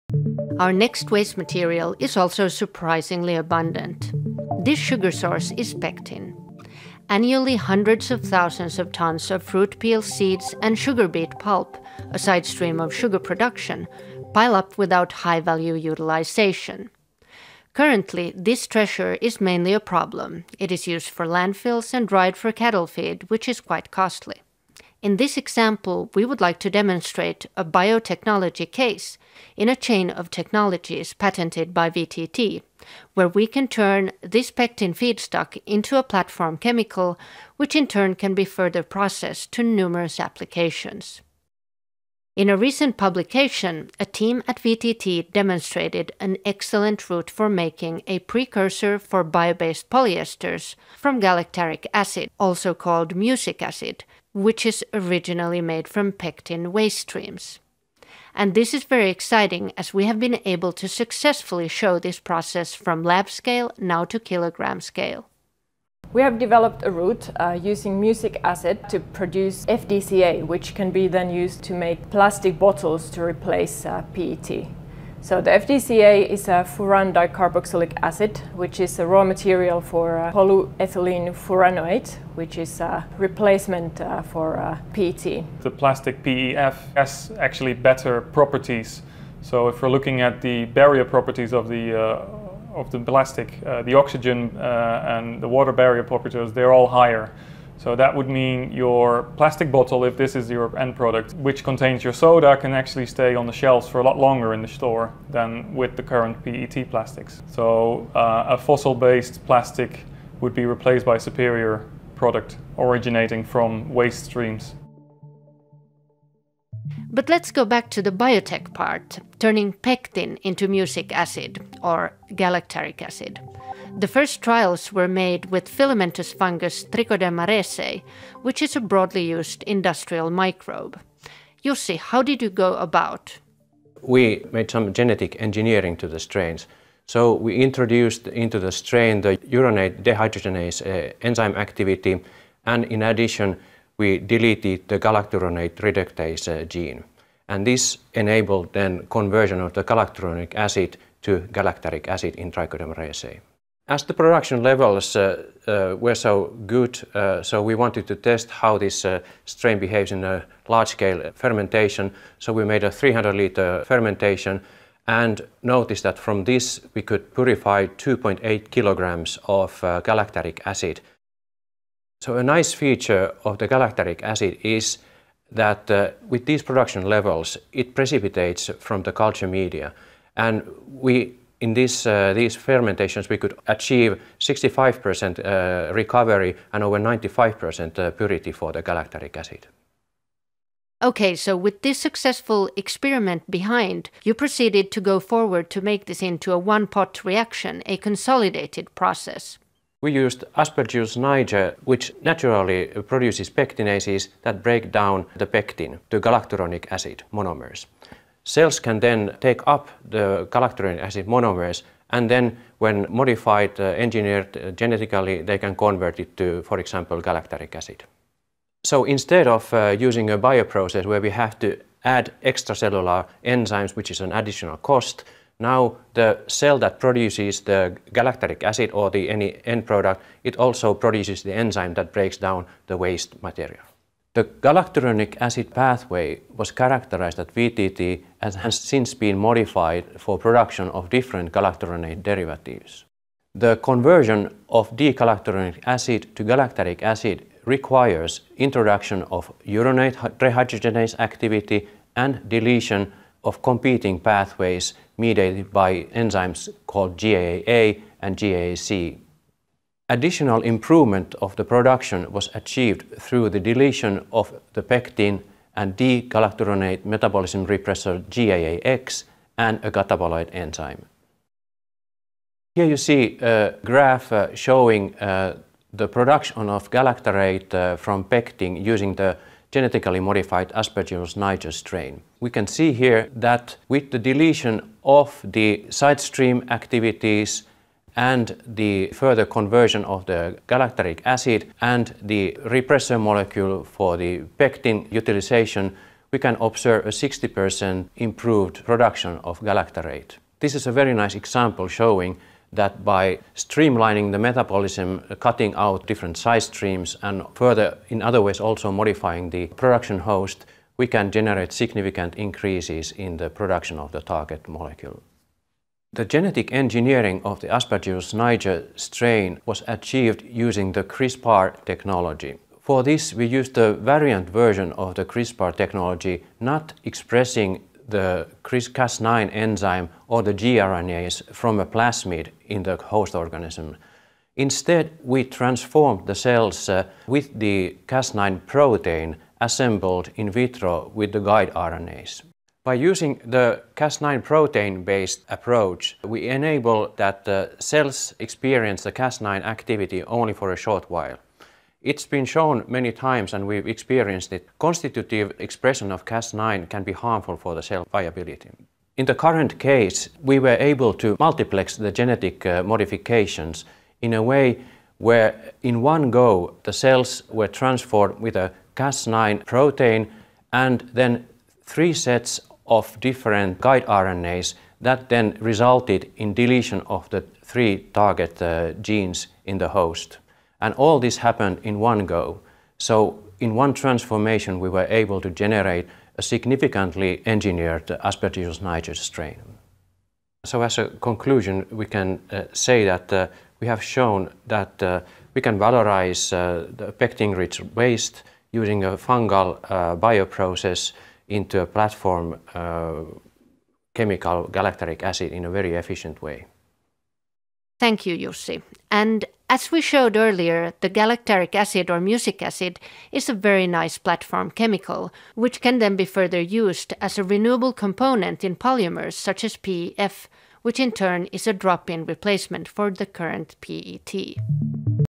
Microbial drop-in replacement for PET from fruit peels
Synbio Powerhouse Annual Seminar: Why the Bioeconomy needs Synthetic Biology

Jussi Jäntti, Principal Scientist, VTT
Sari Rautiainen, Senior Scientist, VTT
Nicolaas van Strien, Research scientists, VTT